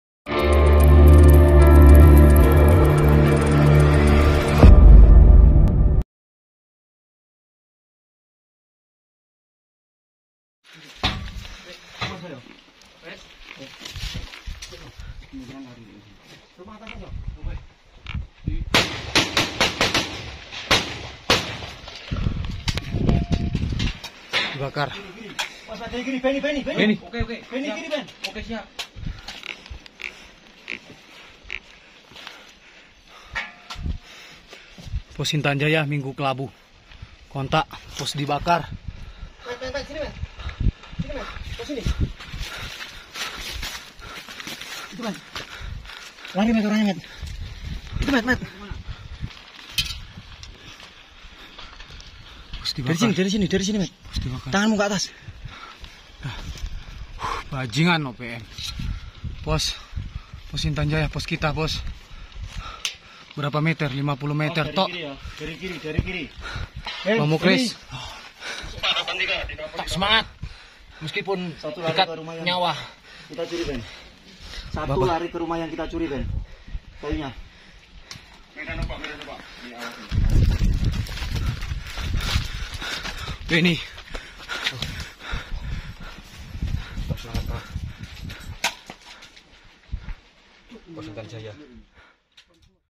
Viral..TNI kontak tembak dngan KKB OPM
Berita terbaru dari tanah papua, sekelompok pengacau keamanan kembali beraksi tapi dengan kesigapan dan kesiapan pasukan TNI, mrk pun mendapatkan perlawanan yang sengit...